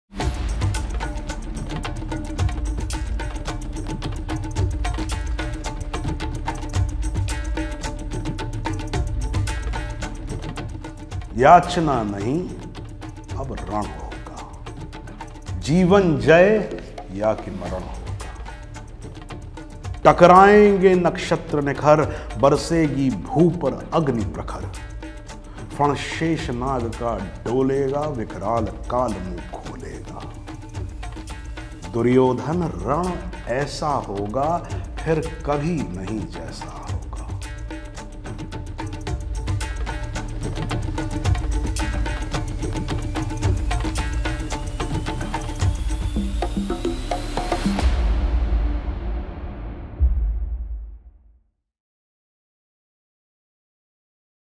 Saurabh Shukla Episode Promo | सौरभ शुक्ला एपिसोड प्रोमो
Saurabh Shukla Episode Promo
सौरभ शुक्ला एपिसोड प्रोमो

नई धारा एकल में जाने-माने अदाकार सौरभ शुक्ला ने रामधारी सिंह 'दिनकर' के खंड-काव्य 'रश्मिरथी' में से एक अंश का पाठ किया।

आपको यह प्रस्तुति कैसी लगी, हमें कॉमेंट्स में ज़रूर बताएँ।

हमसे जुड़ें –

सोशल मीडिया पर फॉलो करें -